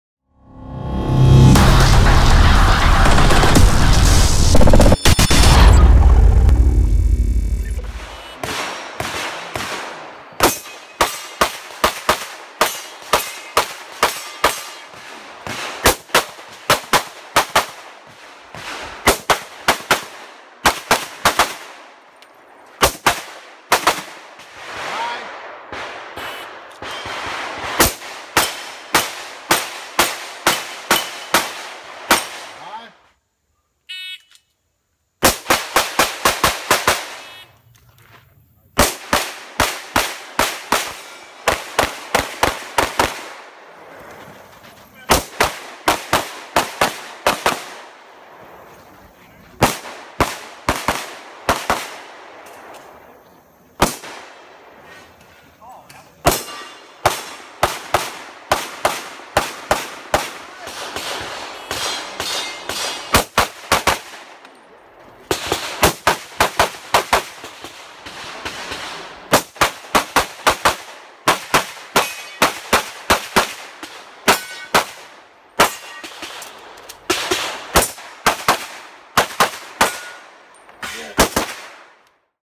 KYLE HOLSCLAW
Akai Team Shooter Kyle Holsclaw shooting at Spartanburgs USPSA monthly match.